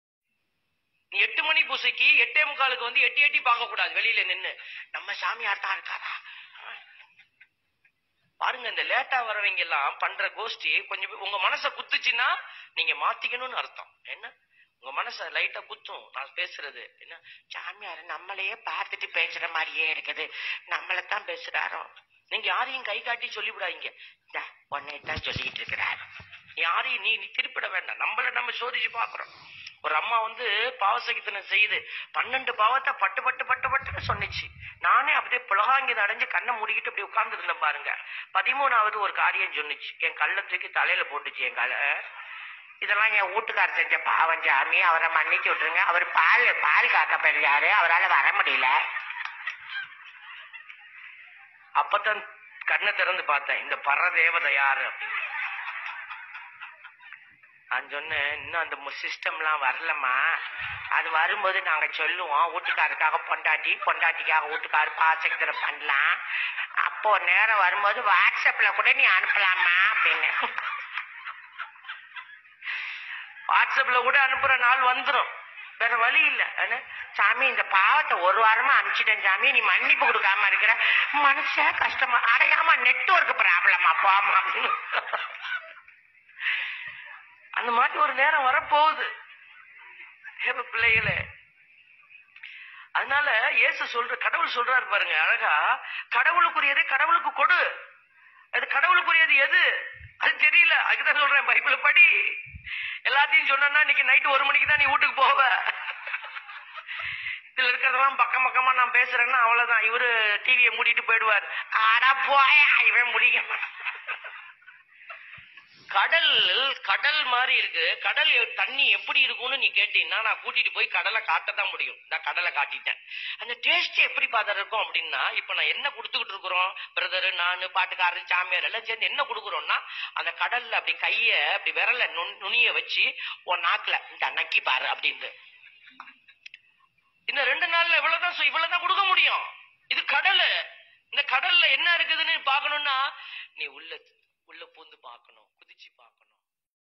பாவமன்னிப்பு ஃபாதர் | comedy father | Tamil Catholic Reformation
*கெளம்பிட்டாய்ங்கய்யா.. _கெளம்பிட்டாய்ங்க_ . 😁*

**காலத்தின் கட்டாயமோ இது, இனியும் பாரம்பரிய முறையில் நிதானமாக அருள்வடிய பேசிக்கொண்டிருந்தால் கூட்டம் மொத்தமாய் வடியும் என்பதை உணர்ந்த போப்பு மார்க்கத்தார் தங்கள் குருமார்களுக்கு பூரண சுதந்திரம் வழங்கிவிட்டது போலும்.. எல்லாம் தந்தை பெர்க்மான்ஸ் விளைவுதானே ? 😁